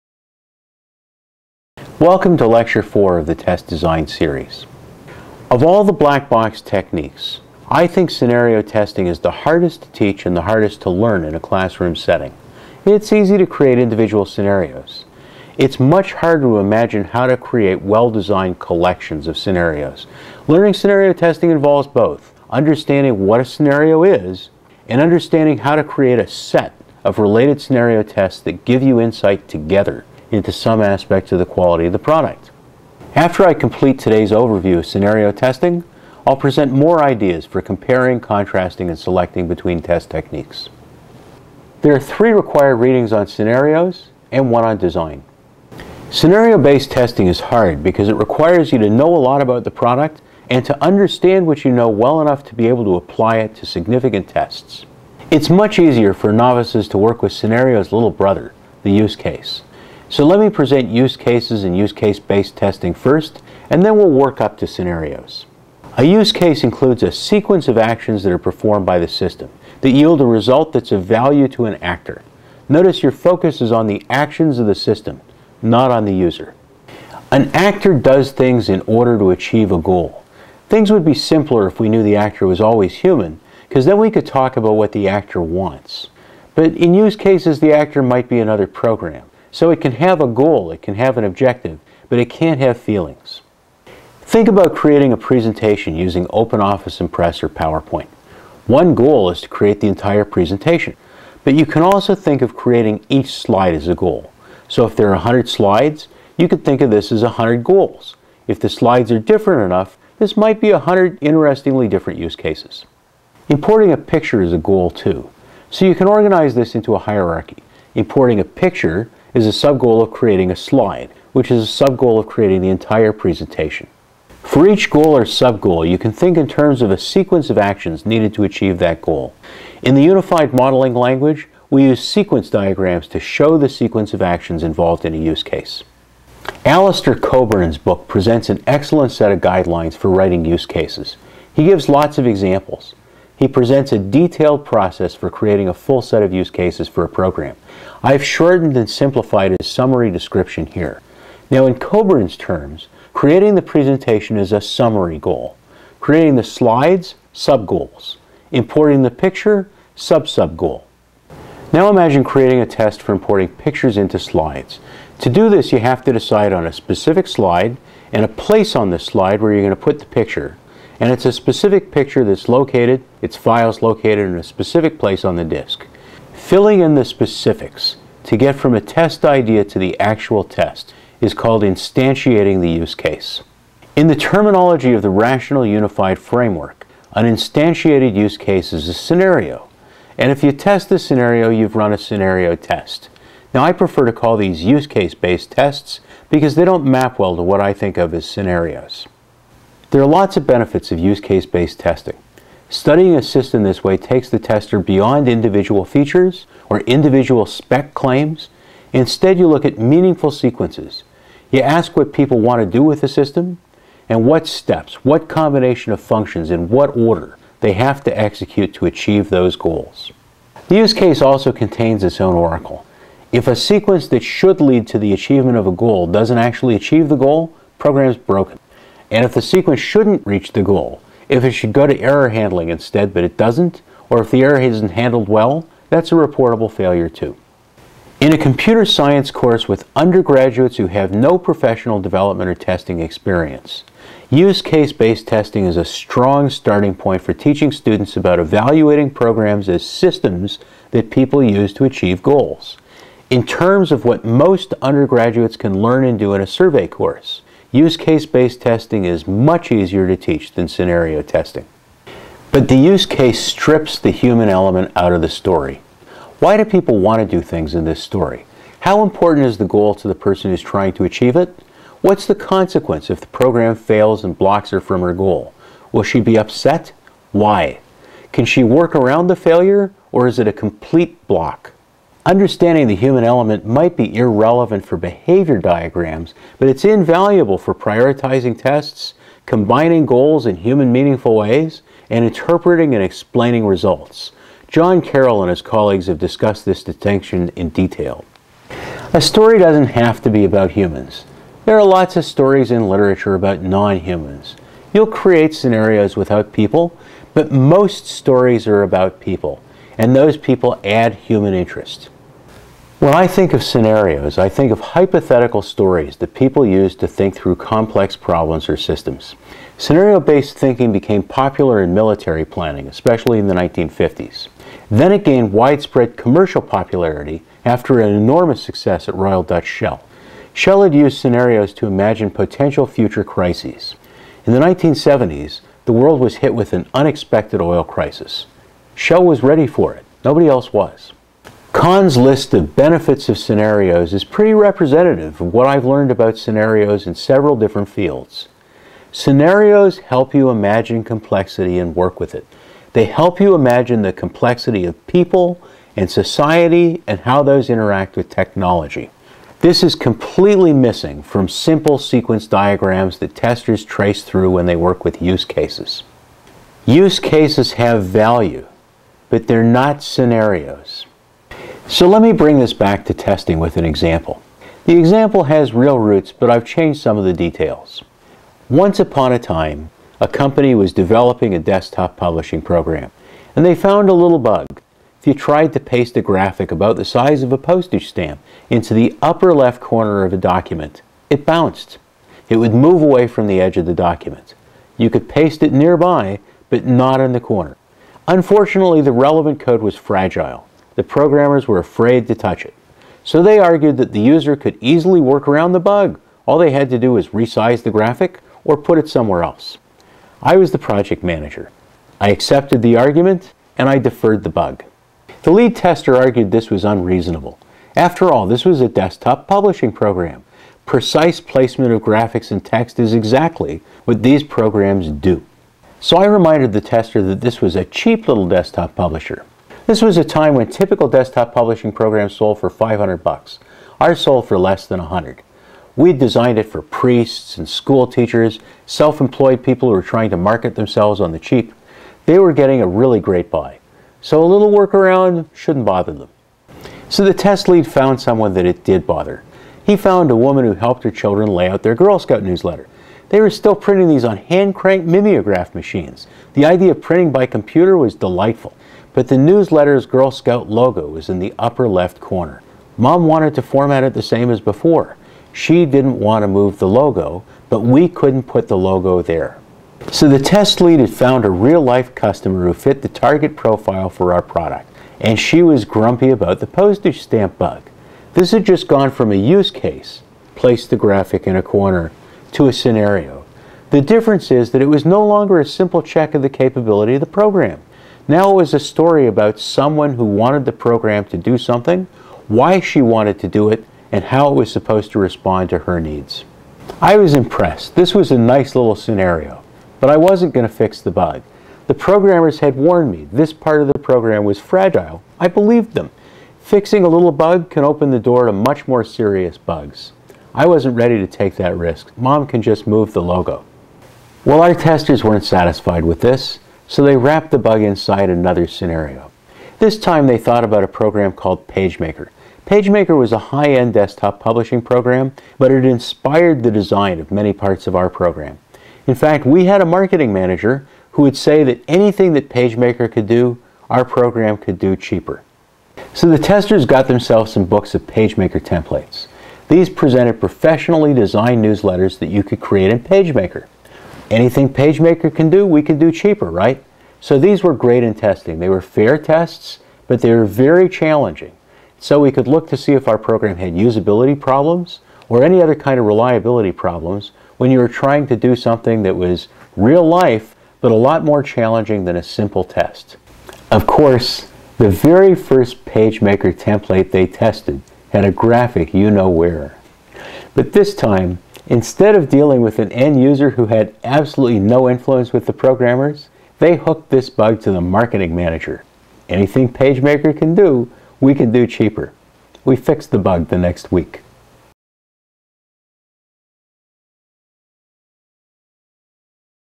BBST Test Design 4A: Scenarios and Use Cases: Comparatively Evaluating Test Techniques
In this lecture, Kaner introduces scenario testing, including 17 approaches to creating suites of scenario tests. He also suggests comparing test techniques in terms of their core strengths and describes 18 potential core strengths.